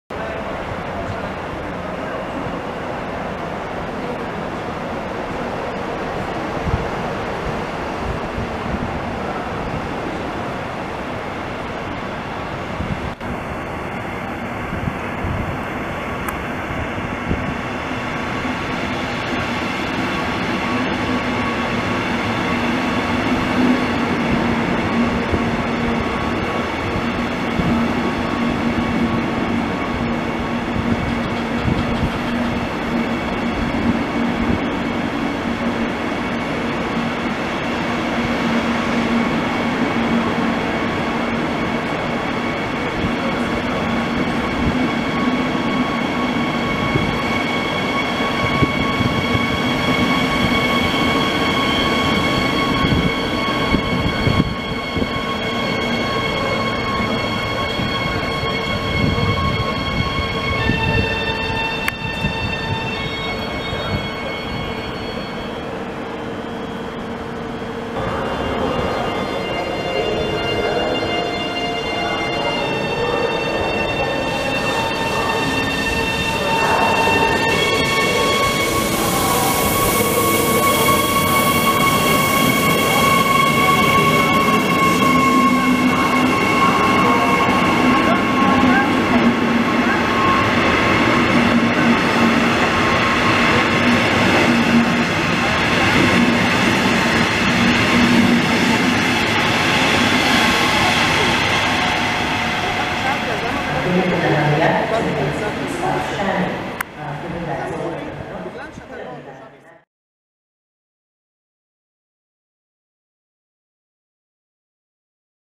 Tel Aviv–Jerusalem railway - רכבת ישראל - Bahn - Zug - Vonat
At Ben Gurion station. The Tel Aviv–Jerusalem railway (also high-speed railway) is a railway line that will connect the cities of Tel Aviv and Jerusalem in Israel.